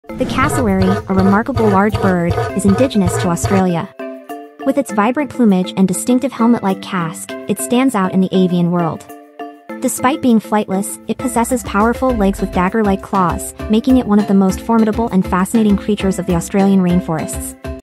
Meet the Majestic Cassowary: Australia's Fascinating Flightless Bird
Join us on a captivating journey as we delve into the world of the cassowary, a magnificent and enigmatic bird found in the lush rainforests of Australia. Discover its incredible features, unique behavior, and the crucial role it plays in its ecosystem. Get ready to be amazed!